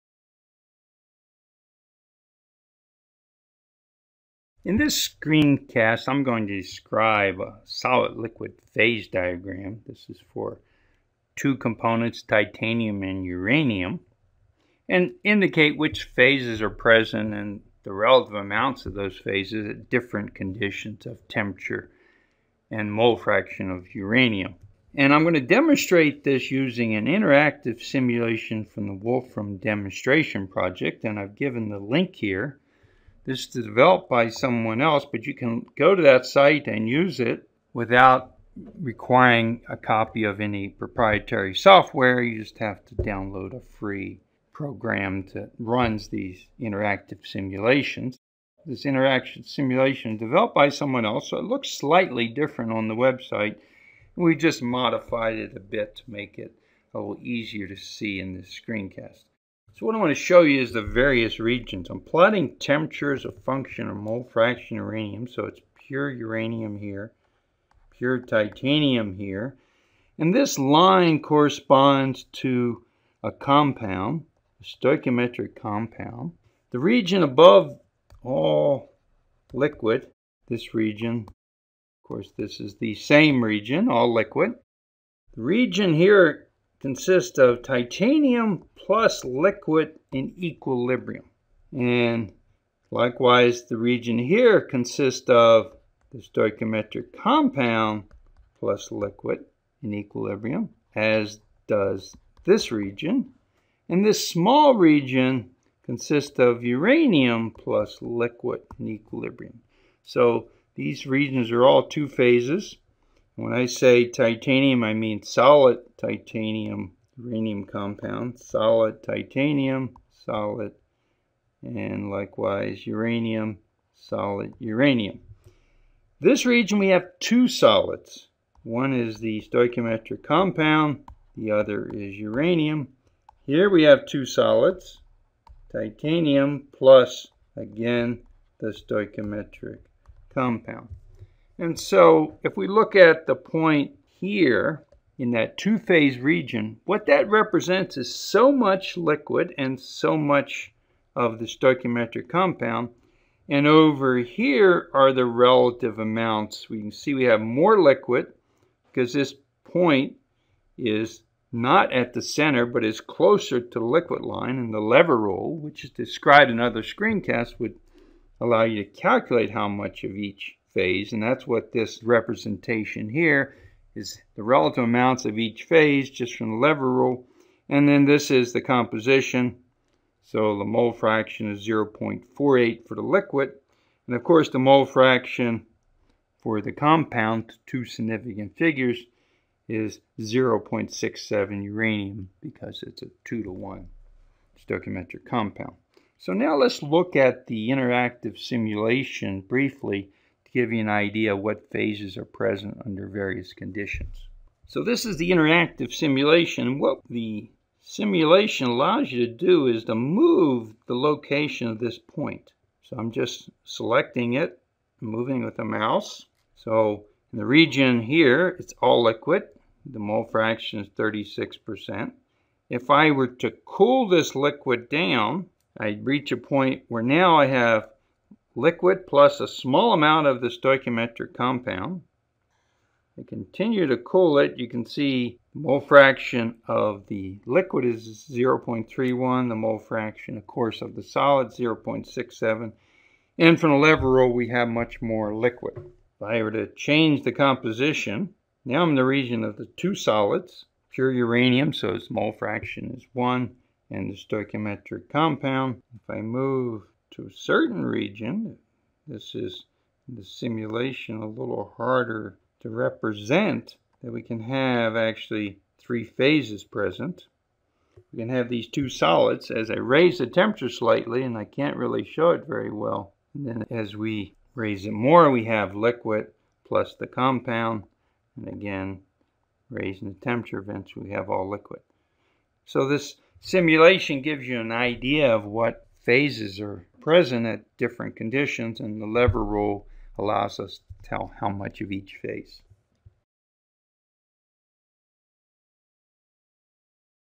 Solid-Liquid Phase Diagram (Interactive Simulation)
Organized by textbook Describes the phases present on a temperature versus mole fraction diagram for the titanium-uranium system using an interactive Mathematica simulation. Made by faculty at the University of Colorado Boulder, Department of Chemical & Biological Engineering. Reviewed by faculty from other academic institutions.